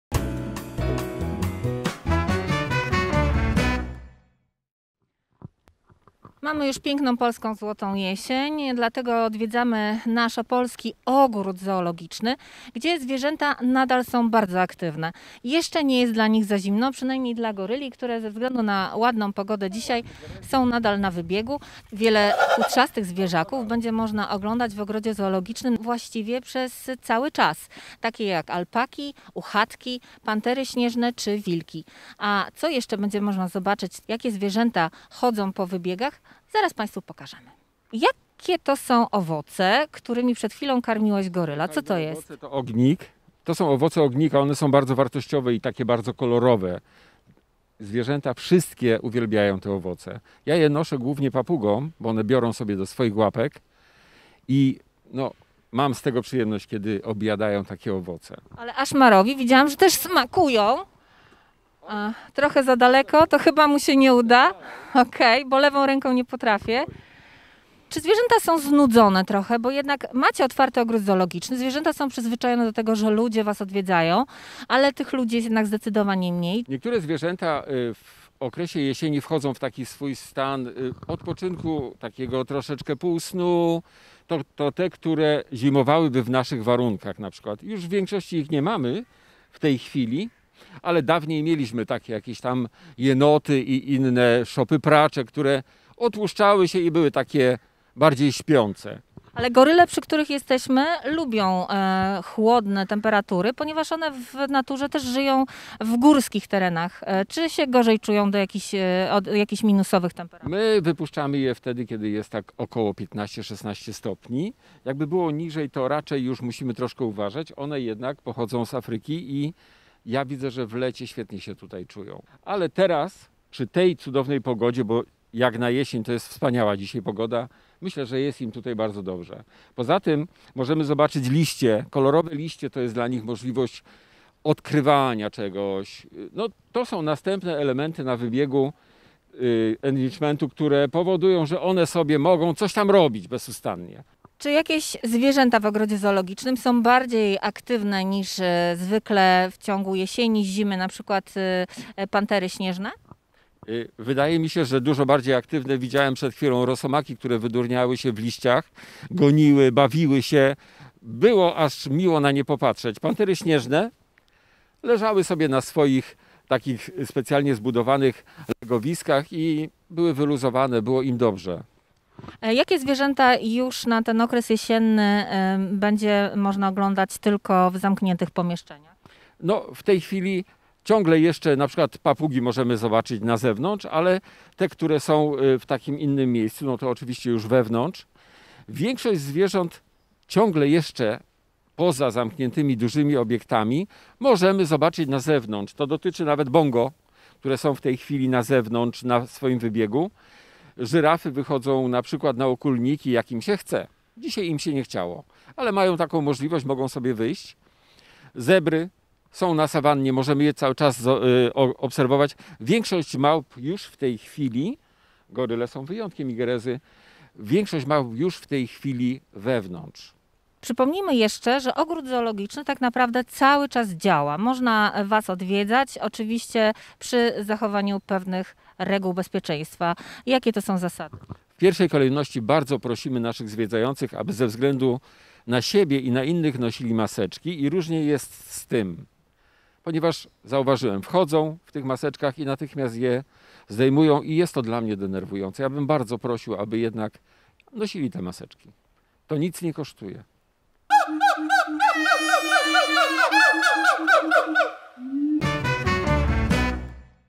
Na czasie - Opolskie Zoo Jesienią
Opolski Ogród Zoologiczny cały czas pozostaje otwarty dla zwiedzających. Spadające liście dają dużą frajdę czerwonym pandom czy gorylom. Warto odwiedzić ZOO Opole zwłaszcza w jesiennym okresie.